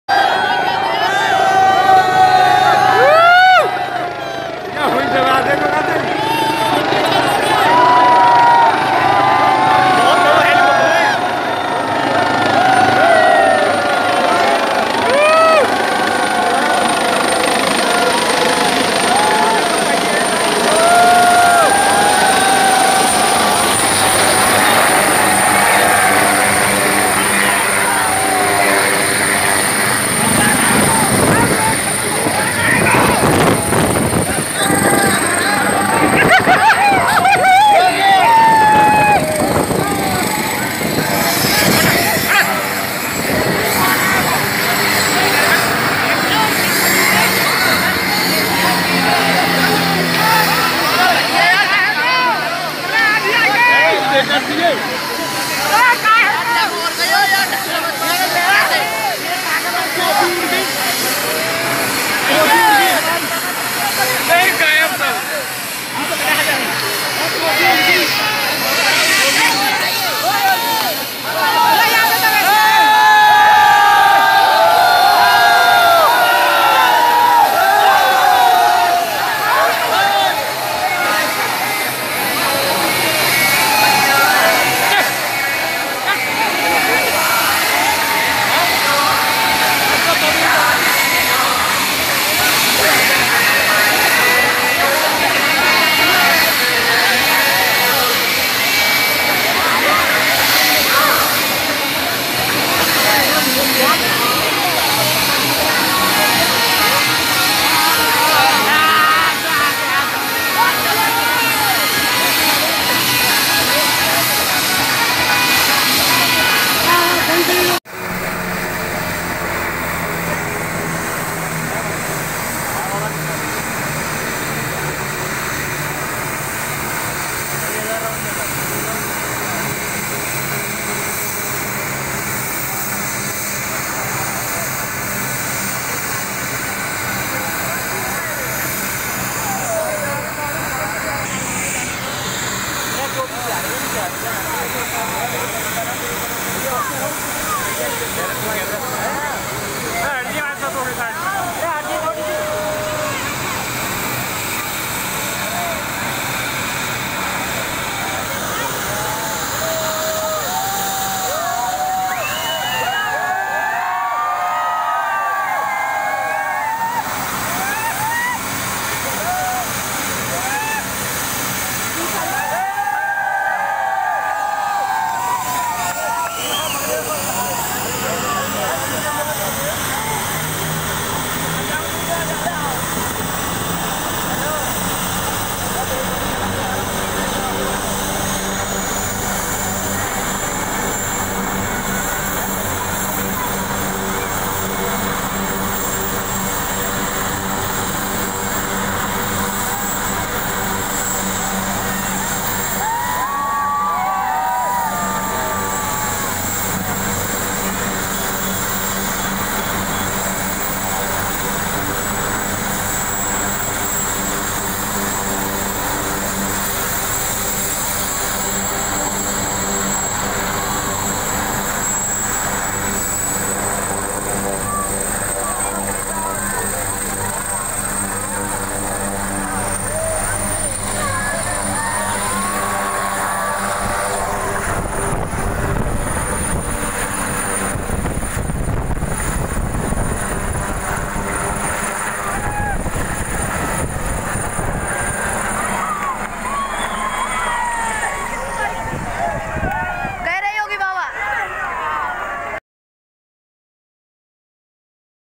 हेलिकॉप्टर से रबूपुरा पहुँचे योगी आदित्यनाथ | Yogi Rabupura helicopter Landing / Takeoff jewar Noida
5 साल में पहली बार आये।चुनाव प्रचार मे